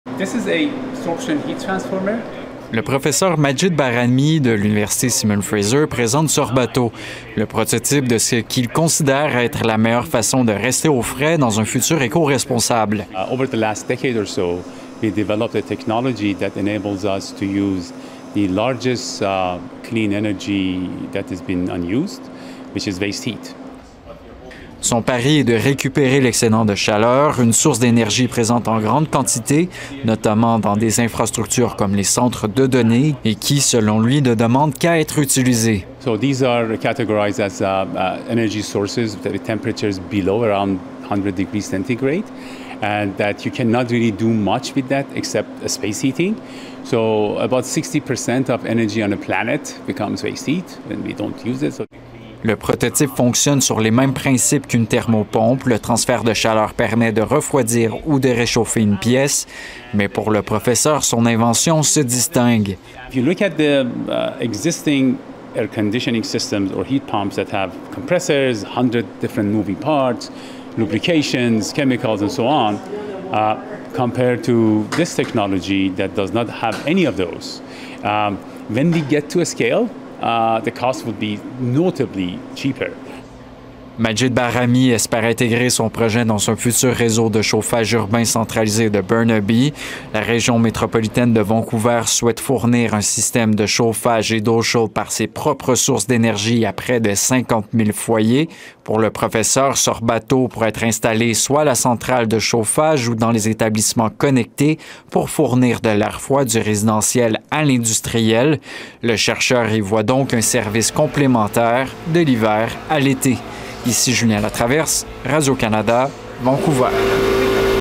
Refroidir des maisons en récupérant de la chaleur
Des chercheurs de SFU proposent une façon plus écologique et économique pour refroidir des maisons. Le prototype utilise le même principe qu’un système de pompe à chaleur, sans utiliser d'électricité ou générer d'émission.

Un reportage de Julien Latraverse.

Lisez notre article « L'été a-t-il été anormalement chaud chez vous? » pour plus de détails sur la chaleur au Canada en 2025